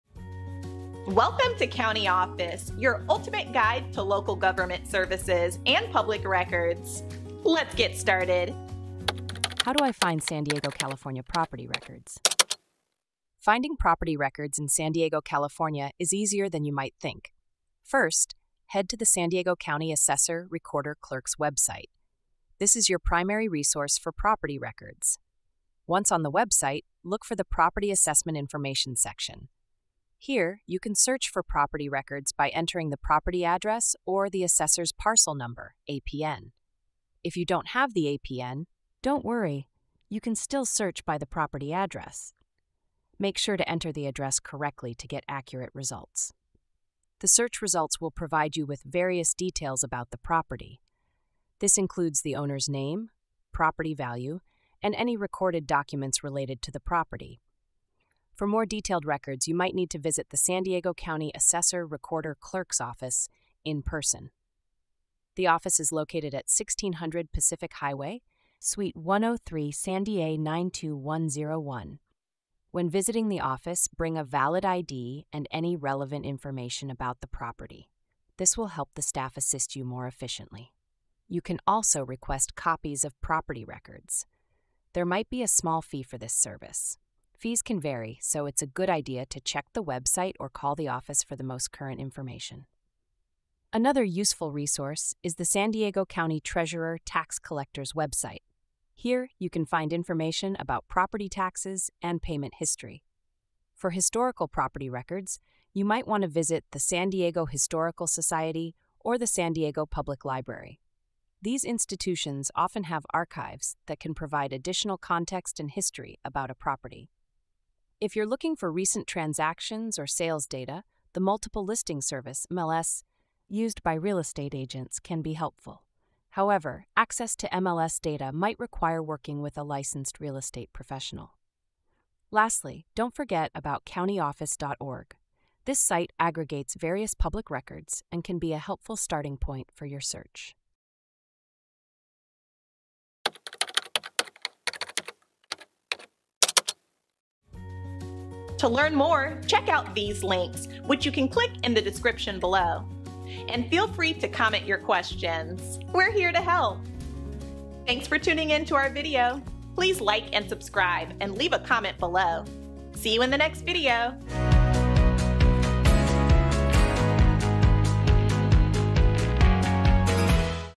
How Do I Find San Diego, California Property Records? - CountyOffice.org
How Do I Find San Diego, California Property Records? Have you ever needed to find property records in San Diego, California? In this video, we'll guide you through the process of accessing property records in San Diego County. Discover how to search for property assessment information online and learn about the resources available to obtain detailed records. Whether you're a homeowner, real estate professional, or researcher, understanding how to access property records can be invaluable.